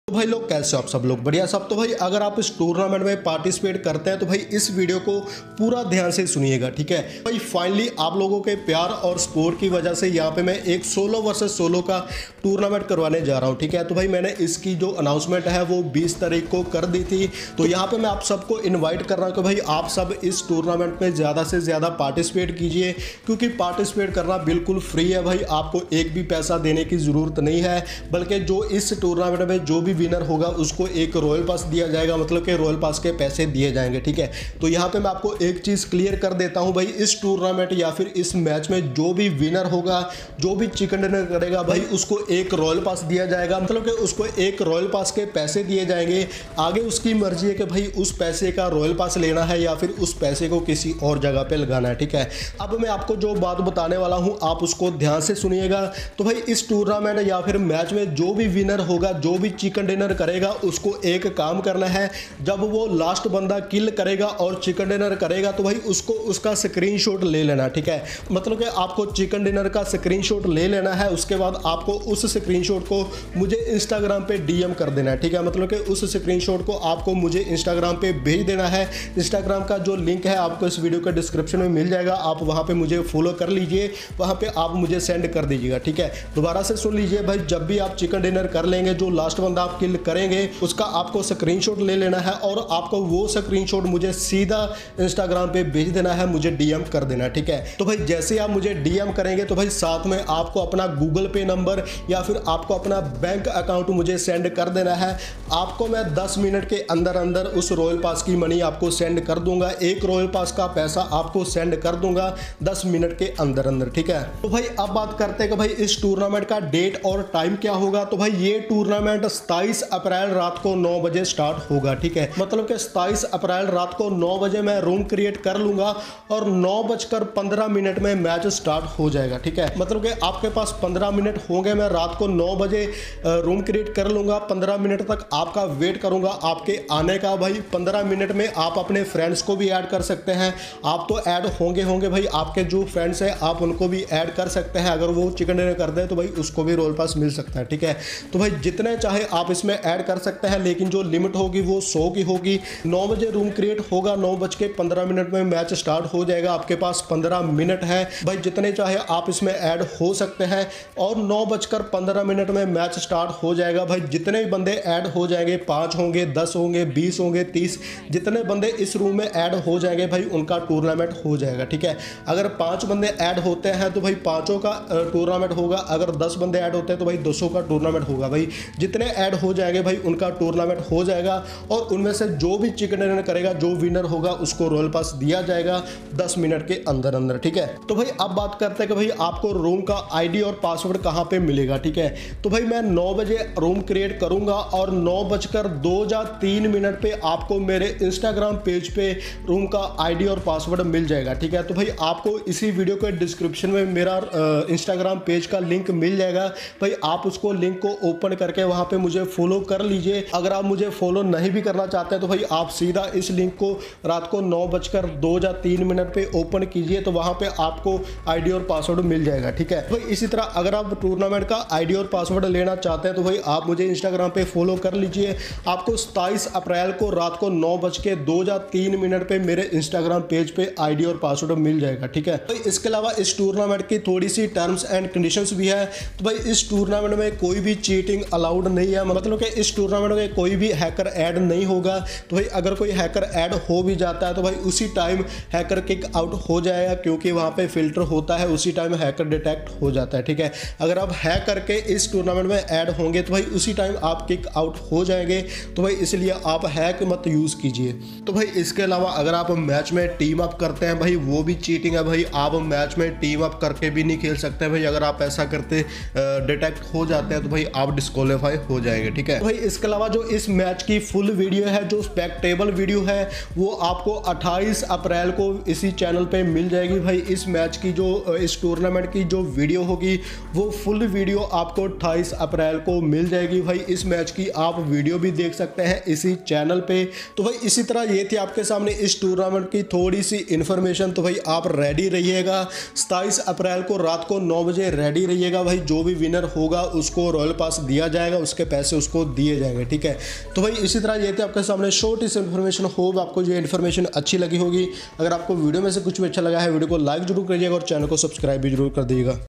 A GIFT FOR YOU GUYS | FINALLY SOLO VS SOLO TOURNAMENT IS HERE | WINNER PRICE 1 ROYAL PASS = 753रू
Jab ap tournament me chiken diner kr lenge to ap ko uska screenshot le lena hai or mujhe Instagram send kr dena hai

Or sath me apko apna Google pay number ya fir bank account details send kr dena hai